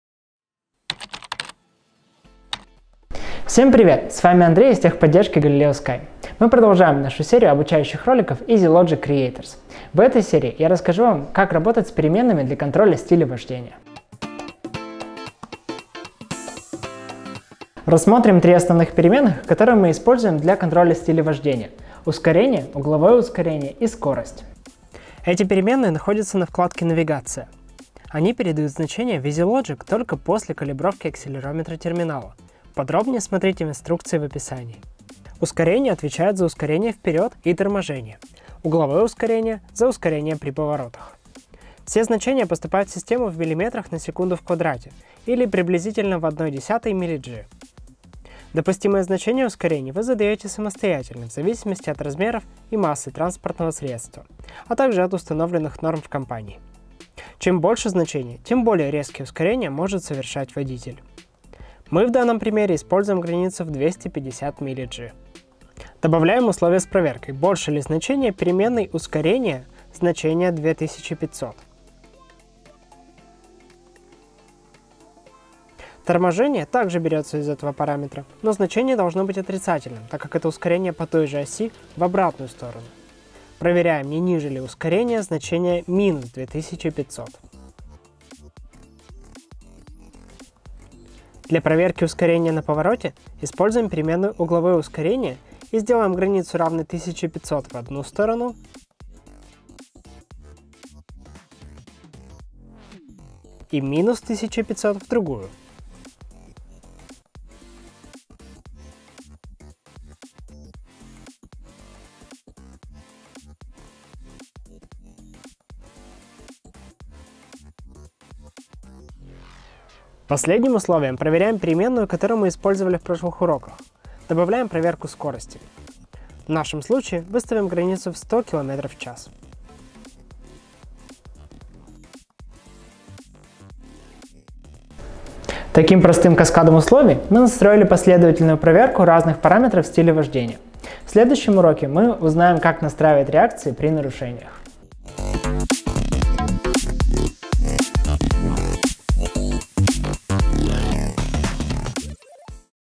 Ускорение, угловое ускорение и скорость | Easy Logic Creators | Урок 3.1
В этом уроке мы расскажем, как контролировать стиль вождения с помощью установки порогов ускорения, углового ускорения и скорости.